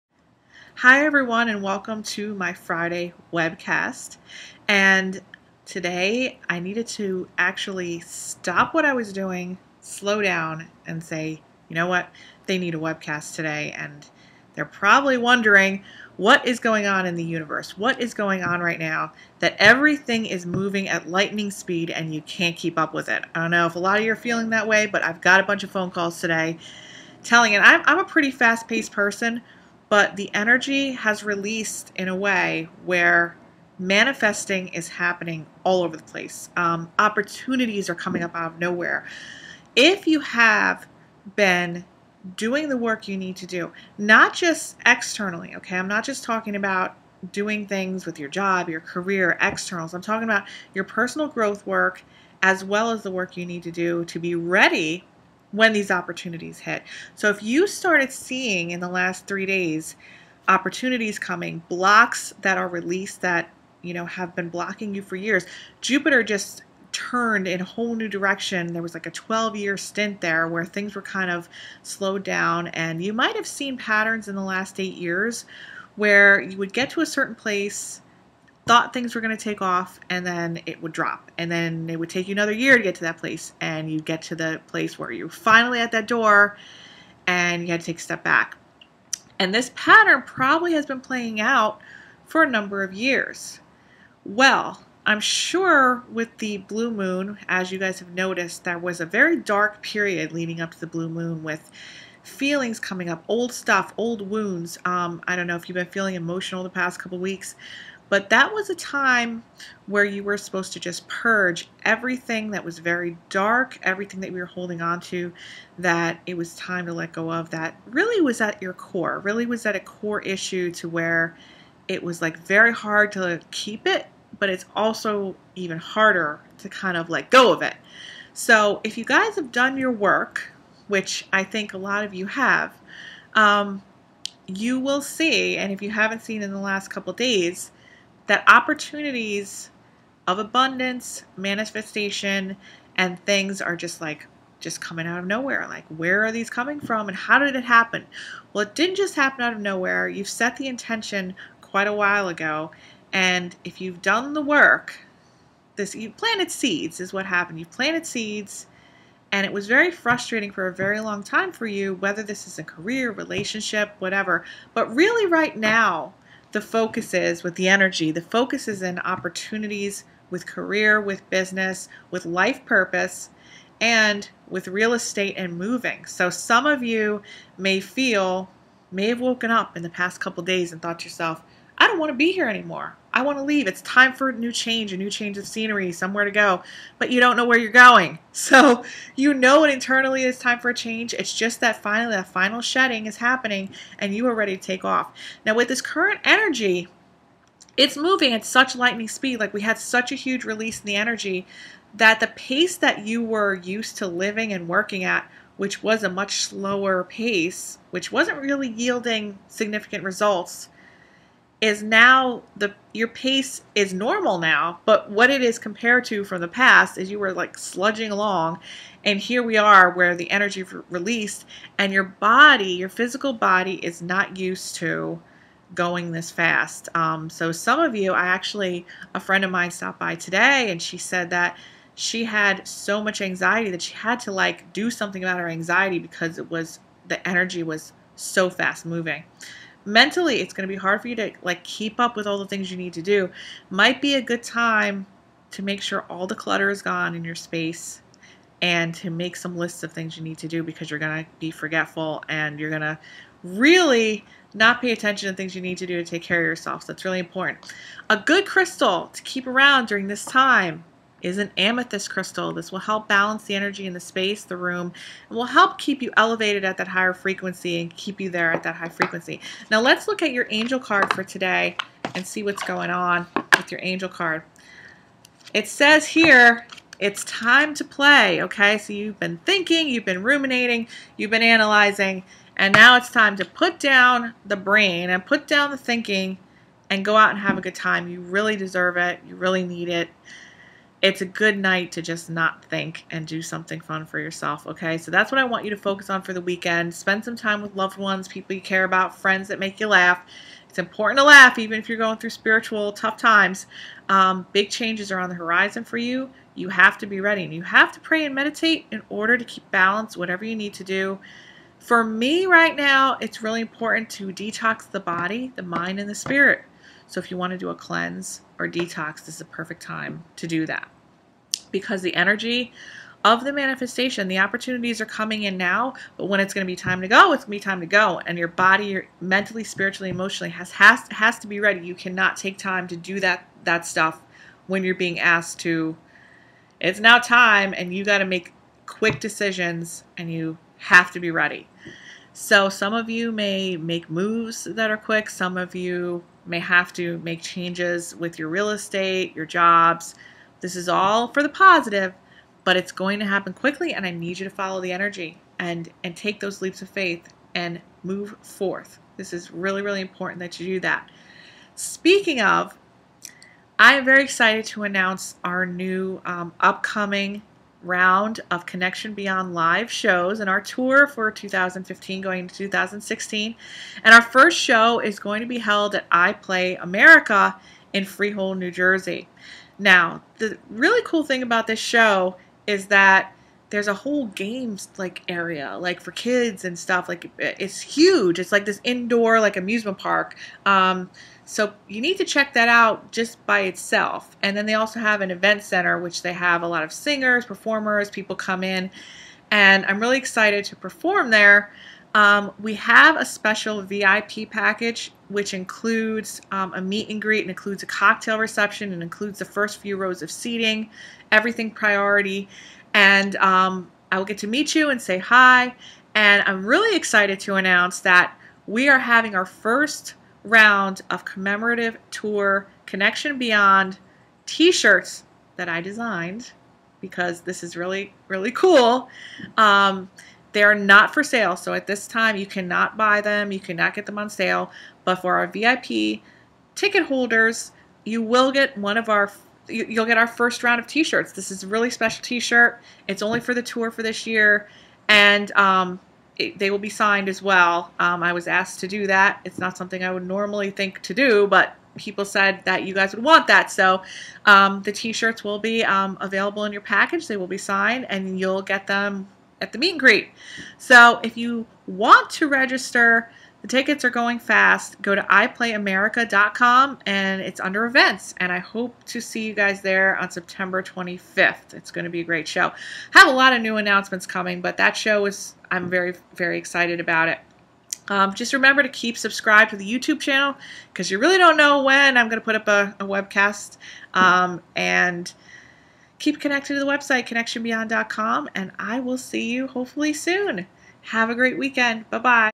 Medium Marisa Liza Pell: Friday Webcast & Weekend Reading!!!
During this weekend webcast and reading, Marisa explains the HUGE release in the energies that are designed to catapult you forward towards your highest dreams. goals and aspirations!!! Do not delay! Take a look at this video and see what's in store for you and how to remain centered throughout this fast paced time!! Enjoy your weekend!